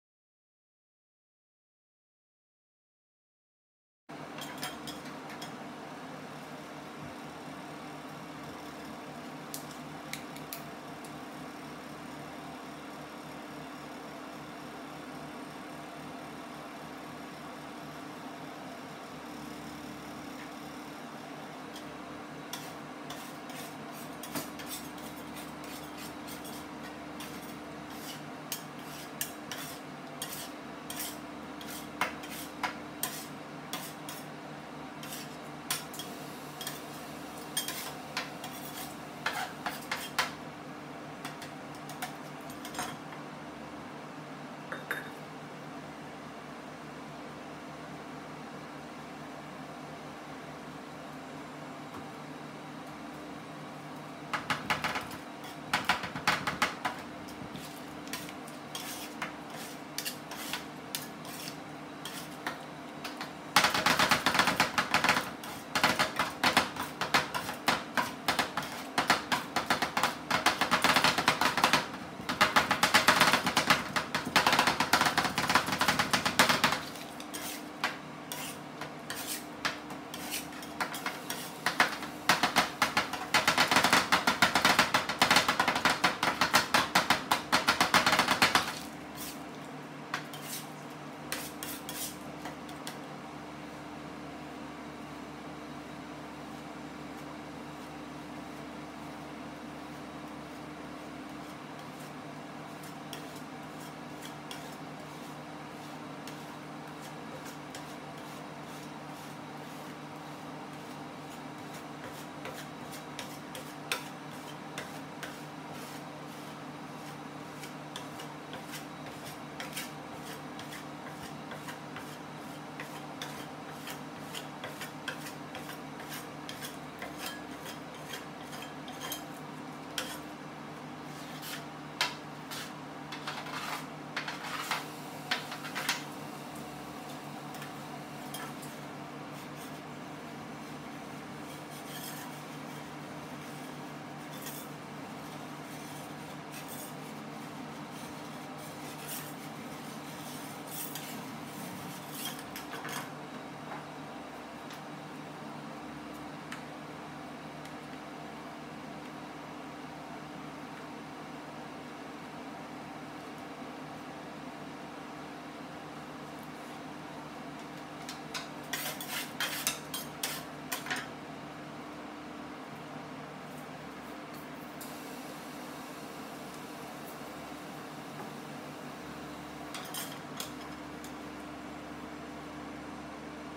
ICE CREAM ROLLS with sports drink "POWERADE" | How to make Thai ice cream roll | ASMR
We prepare fried roll ice cream  with sports drink "POWERADE".
Ice cream recipe as in Thailand.
More interesting videos on our channel MAD ICE CREAM ROLLS